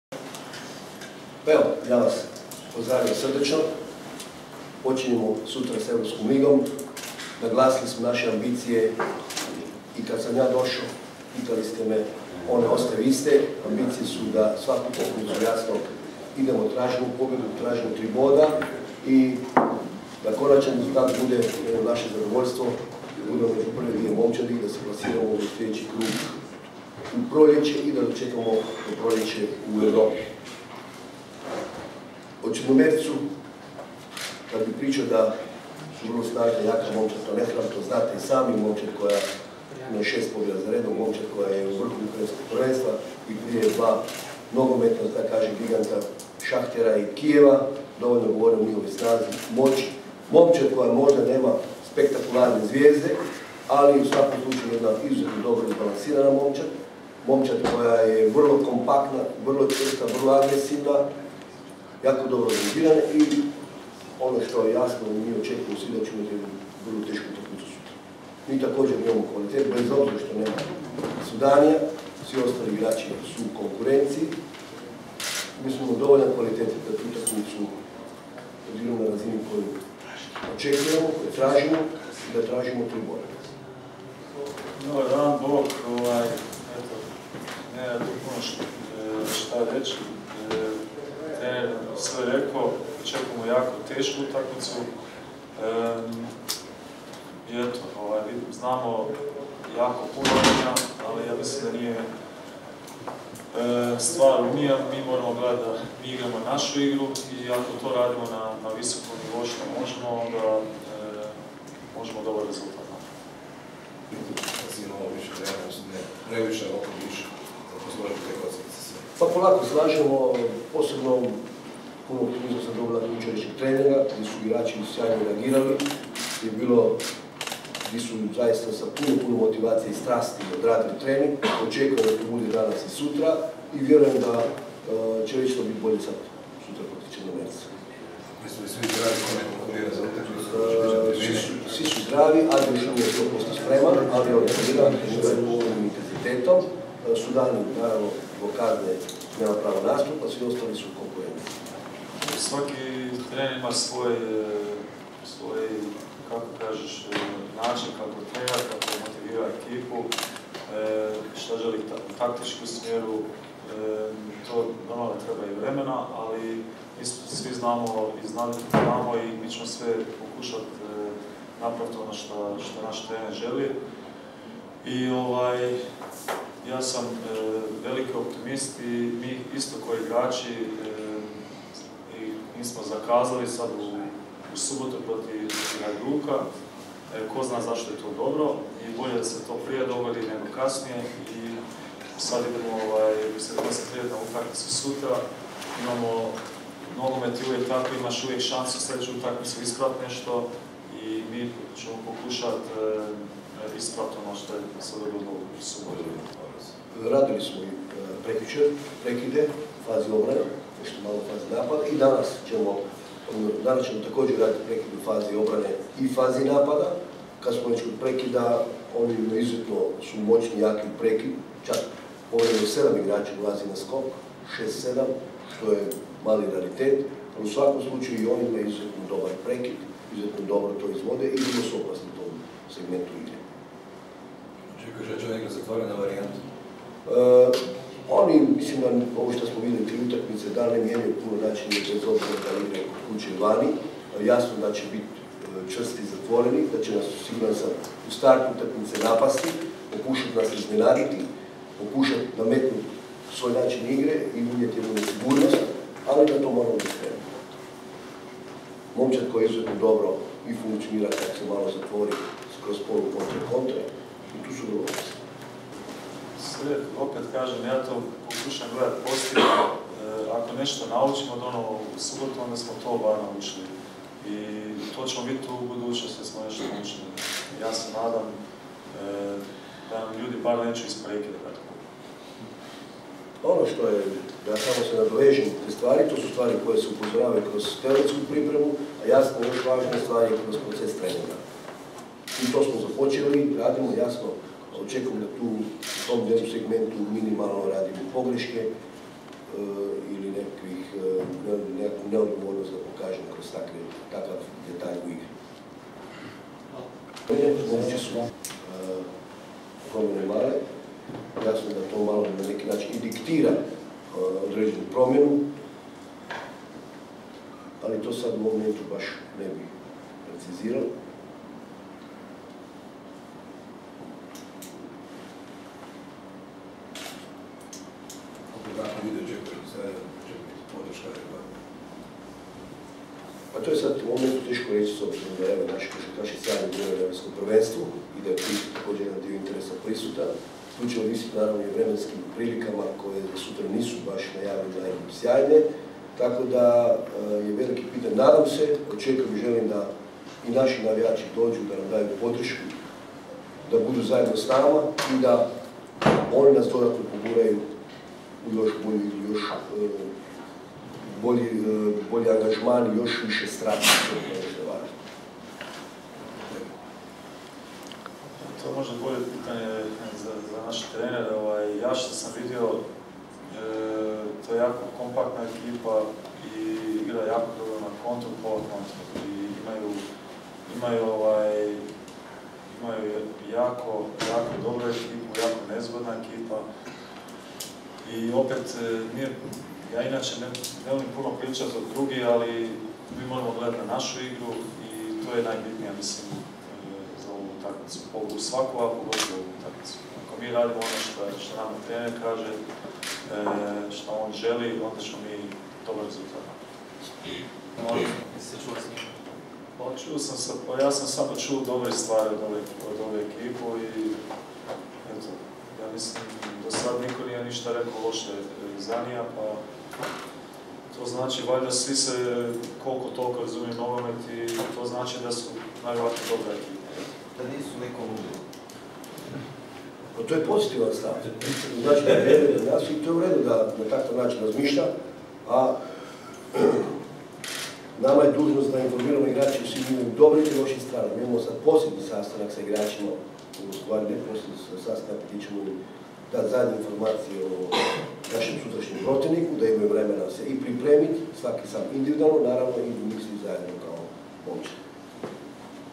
Trener Ivanković i kapetan Šimunić: Najava utakmice s Černomorecom, 18.9.2013.
Trener Dinama Branko Ivanković i kapetan Josip Šimunić najavili su na konferenciji za medije europsku utakmicu s Černomorecom.

18. rujna 2013.